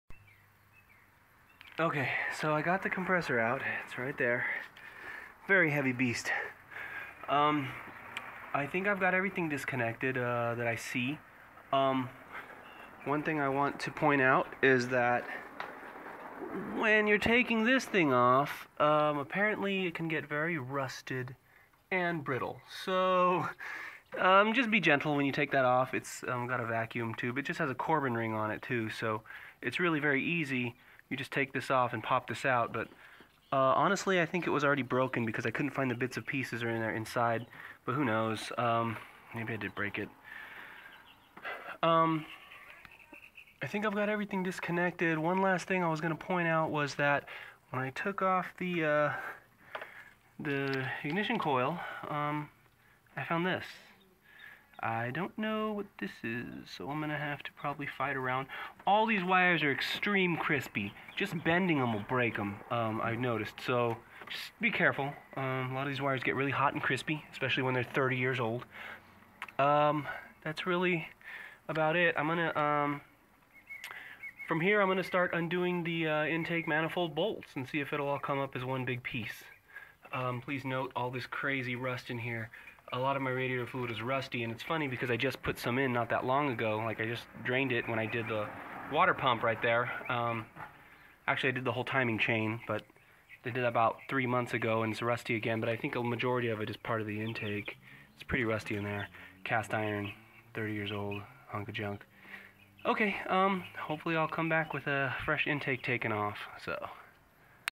Chrysler 360 Intake Manifold Removal 5 of 10
When I say "It goes to a vaccum line" I actually mean "it goes to the exhaust"

That wire that I found, I was able to remove completely and it didn't seem to go anywhere.  Someone told me that it might have gone to a tachometer at some point in its life? Maybe not.

PLEASE SEE ALL VIDEOS BEFORE YOU ATTEMPT TO DO THE SAME THING!!

I learned alot of things that you might want to be aware of before you begin.
check out PART 6 for continuing progress.